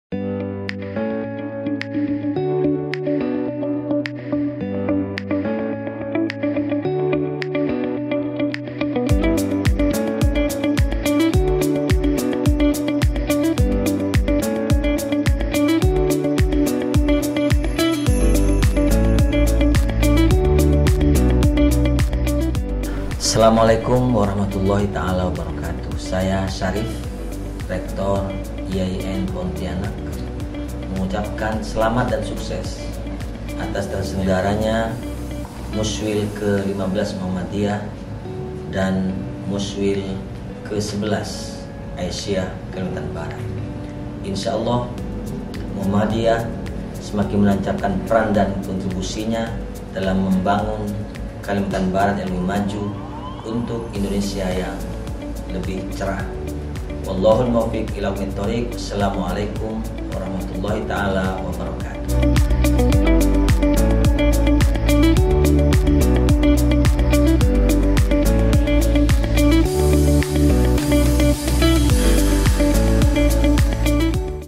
Selamat dan Sukses Atas Muswil ke-15 Muhammadiyah dan Aisyiyah ke-11
Rektor dan Civitas Akademika IAIN Pontianak mengucapkan selamat dan sukses atas terselenggaranya Musyawarah Wilayah (MUSWIL) Muhammadiyah  ke 15 dan Aisyiyah ke 11 Kalimantan Barat.